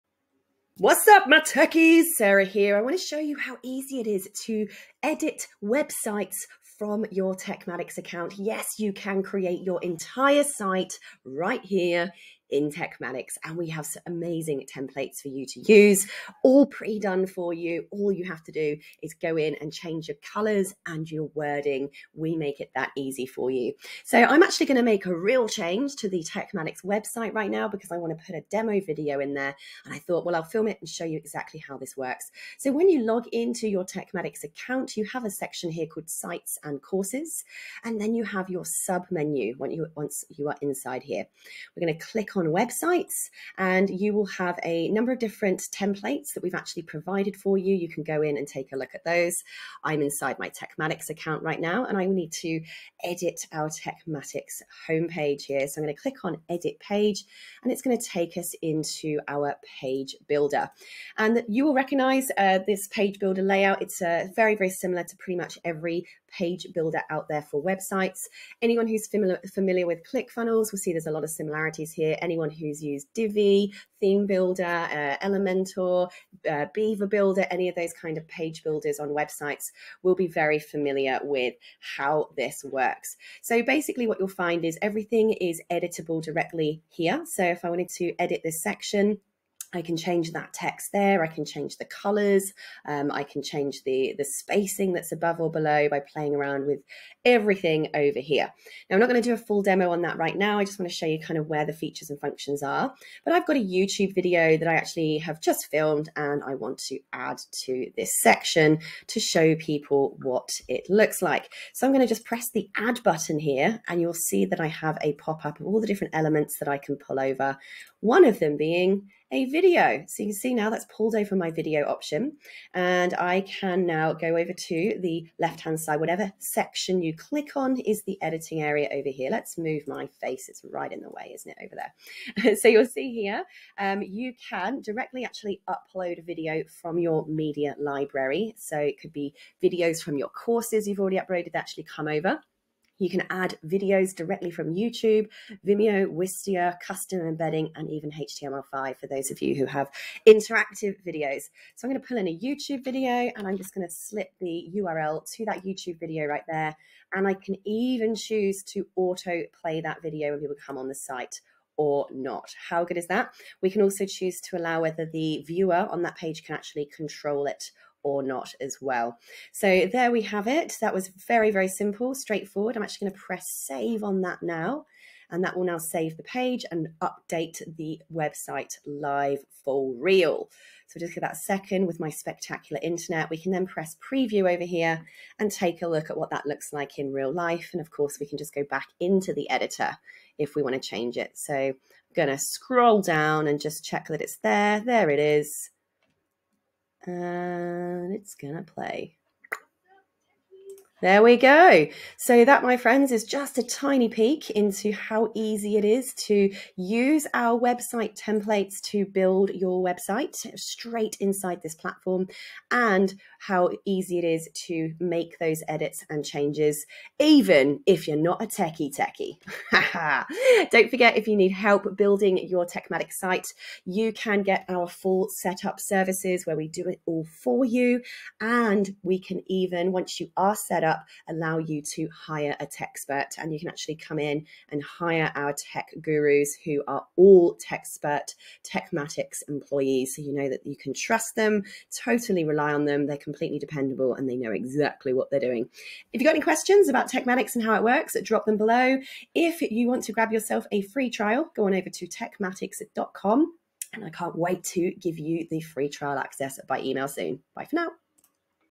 Build Your Website in TekMatix
What's up, Tekkies!

Did you know that TekMatix is also your whole website?!
AND that we have pre-built a selection of websites FOR YOU that you can suck straight into your account?!

No more paying thousands of dollars for complicated website building!!
Bye bye expensive web developers!!

Check out this video of just how easy it is edit your website pages in TekMatix!